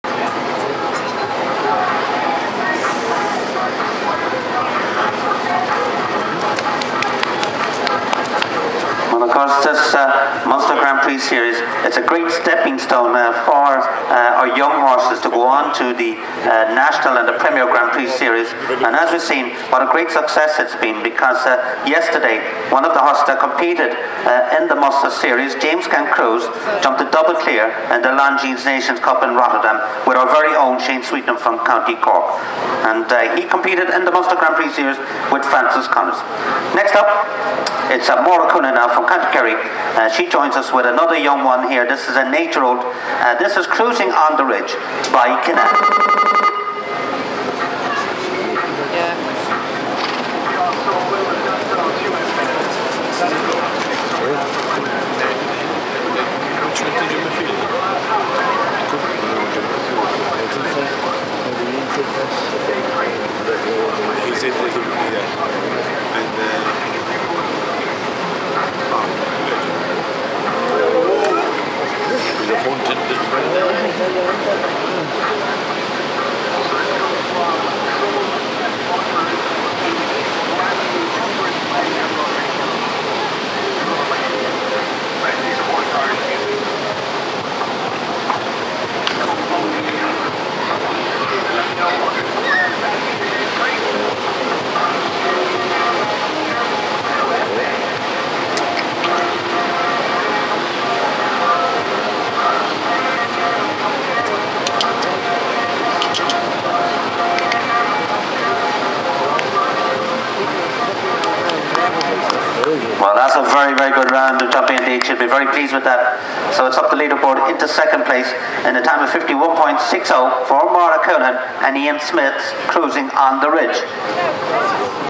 Jump Off 04 Maura Counihan Cruising On The Ridge
Connolly's Red Mills Munster 1.35 Grand Prix 2022 From Charleville Show